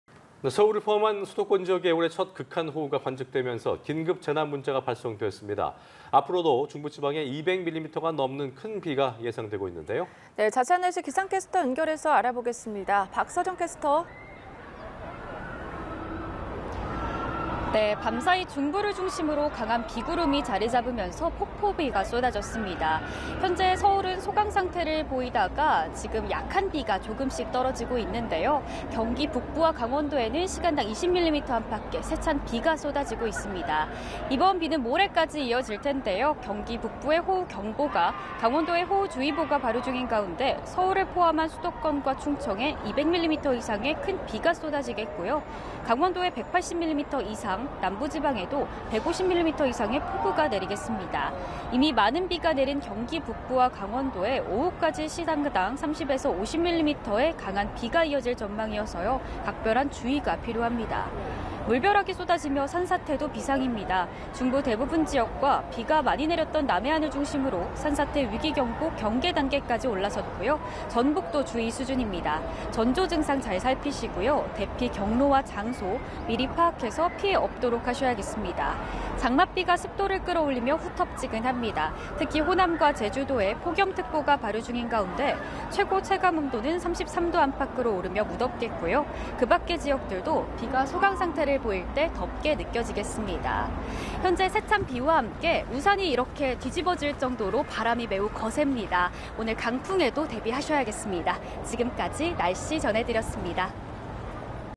[날씨] 수도권 올해 첫 극한 호우…전국 산사태 유의 / 연합뉴스TV (YonhapnewsTV)
[날씨] 수도권 올해 첫 극한 호우…전국 산사태 유의

[앵커]

서울을 포함한 수도권 지역에 올해 첫 극한 호우가 관측되며, 긴급재난문자가 발송됐습니다.

앞으로도 중부지방에 200밀리미터가 넘는 큰 비가 예상되고 있는데요.

자세한 날씨 기상캐스터 연결해서 알아보겠습니다.

박서정 캐스터.

[캐스터]

밤사이 강한 비구름이 중부지방을 중심으로 자리를 옮기면서 폭포비가 쏟아졌습니다.

현재 제가 있는 서울처럼 소강상태를 보이는 곳이 많지만, 경기 북부와 강원에는 세찬 비가 내리고 있습니다.

이번 비는 모레까지 이어질 텐데요.

경기 북부에 호우 경보가, 강원도에 호우주의보가 발효 중인 가운데 서울을 포함한 수도권과 충청에 200mm 이상의 큰비가 쏟아지겠고요.

강원도에 180mm 이상, 남부지방에도 150mm 이상의 폭우가 내리겠습니다.

이미 많은 비가 내린 경기 북부에 오후 내내 시간당 30~50mm의 강한 비가 이어질 전망이어서 각별한 주의가 필요합니다.

물벼락이 쏟아지며 산사태도 비상입니다.

중부 대부분 지역과 비가 많이 내렸던 남해안을 중심으로 산사태 위기 경보 '경계' 단계까지 올라섰고요.

전북도 주의 수준입니다.

전조증상 잘 살피시고요.

대피 경로와 장소 미리 파악해 피해 없도록 하셔야겠습니다.

장맛비가 습도를 끌어올리며 후텁지근합니다.

특히 호남과 제주도에 폭염특보가 발효 중인 가운데 최고 체감온도는 33도 안팎까지 오르며 무덥겠고요.

그 밖의 지역들도 비가 소강상태를 보일 때 덥게 느껴지겠습니다.

세찬 비와 함께 오늘 바람도 매우 강합니다.

강풍 대비도 같이 해주셔야겠습니다.

(박서정 기상캐스터)